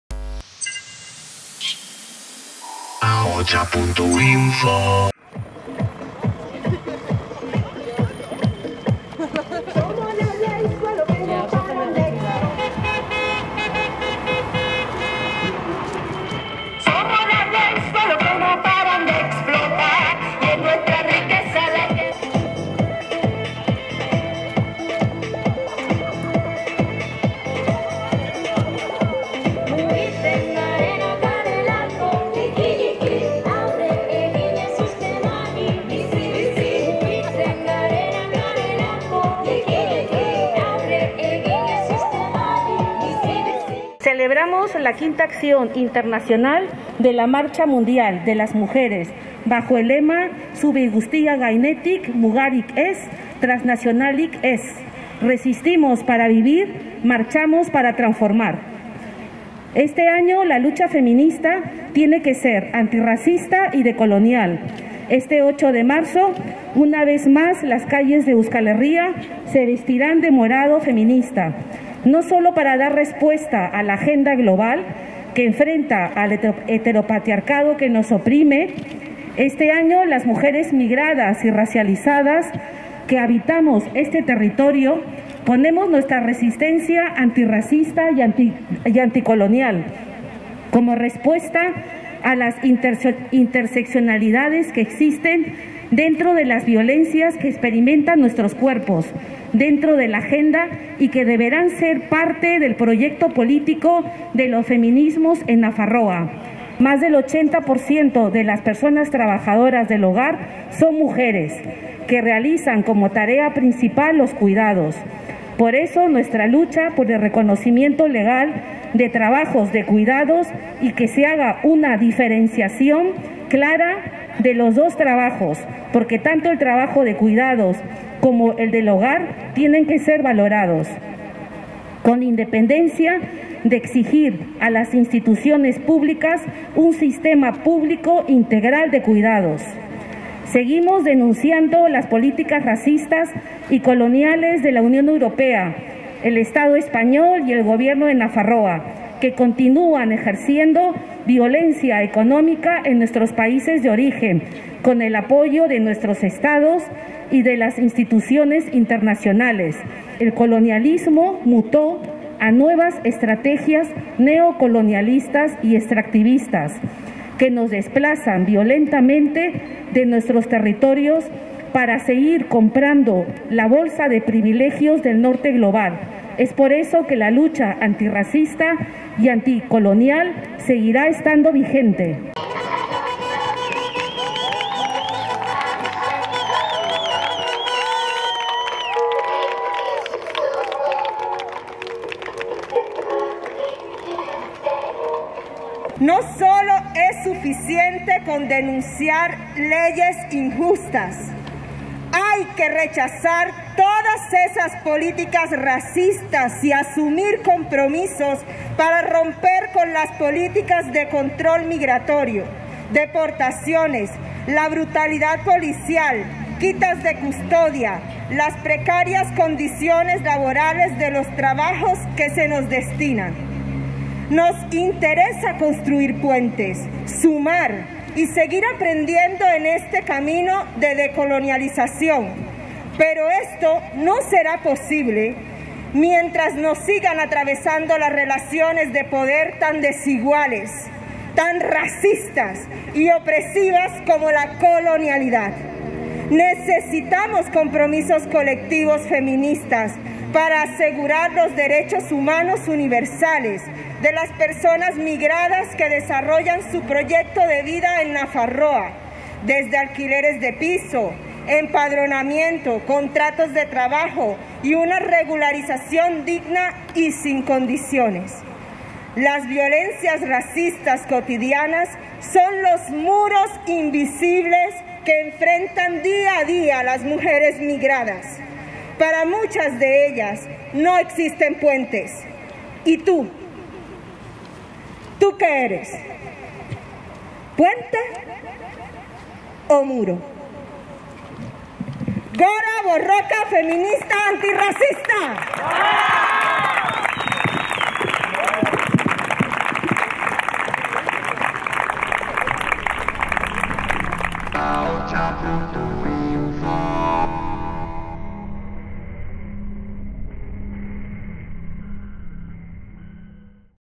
La Marcha Mundial de las Mujeres llega a Iruñea
Bajo el lema “Zubi guztien gainetik mugarik ez, transnazionalik ez. Resistimos para vivir, marchamos para transformar”  la Marcha Mundial de las Mujeres ha llegado a Iruñea. Anuncian que la lucha feminista será antirracista y anticolonial y denuncian que son las mujeres migradas quienes han sacado adelante los cuidados a cambio de sus derechos sociales y laborales.